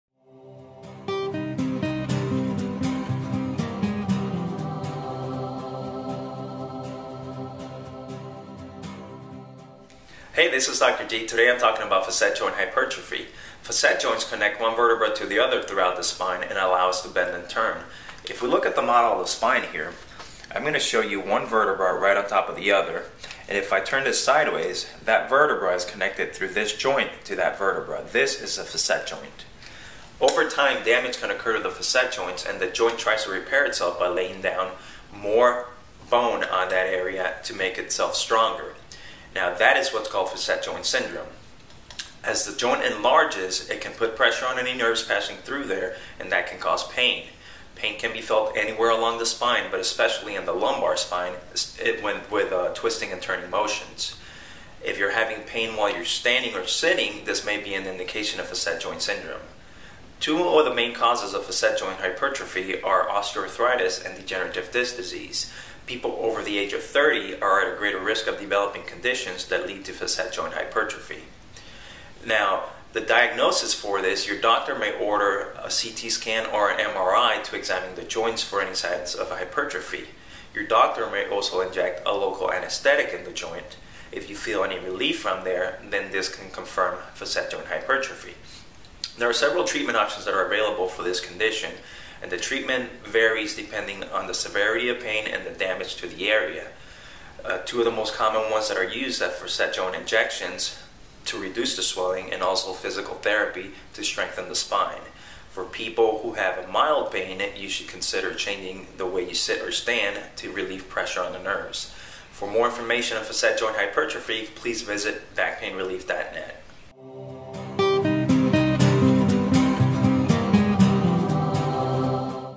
Facet Joint Hypertrophy Explained
Facet joint hypertrophy is a condition affecting the facet joints along the spine. The facet joints help facilitate movement and allow you to bend and turn. When damage occurs to the joint, the joint will attempt to repair itself by adding more bone to the joint. This causes the joint to enlarge creating a condition known as hypertrophy. As the joint enlarges, it puts pressure on the nerves in the spinal cord, which causes pain due to nerve compression. For more information on diagnosis and treatment,